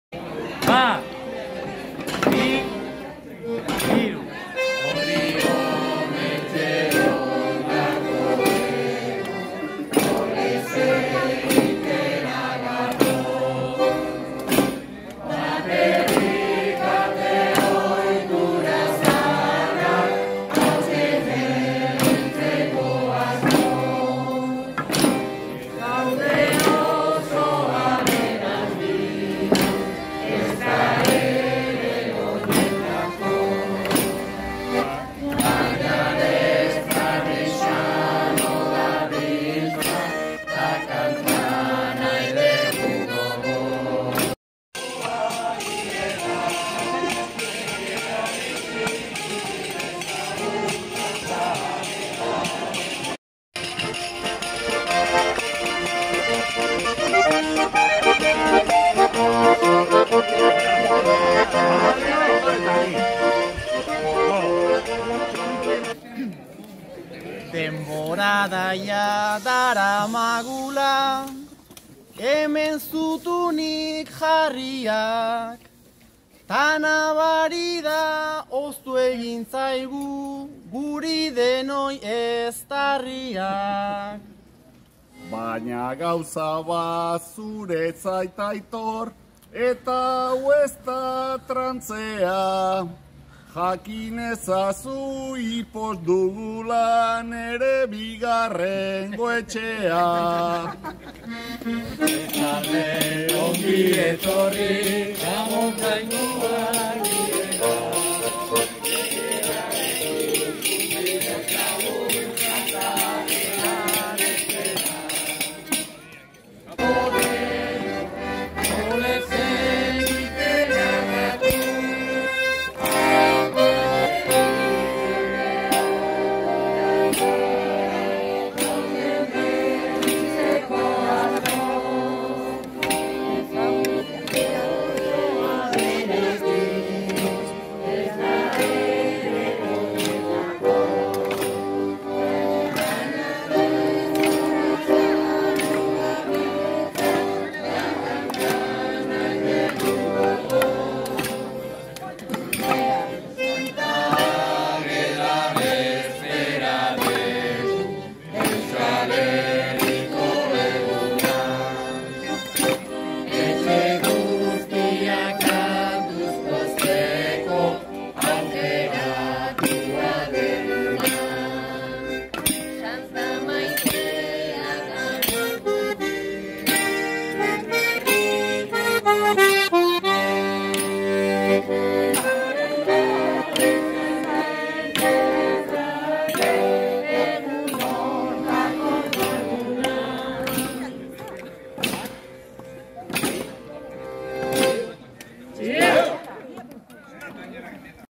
Santa Ageda bezpera Altzan
Irutxulo.hitza.eus | Santa Ageda bezpera ospatu dute Altzan.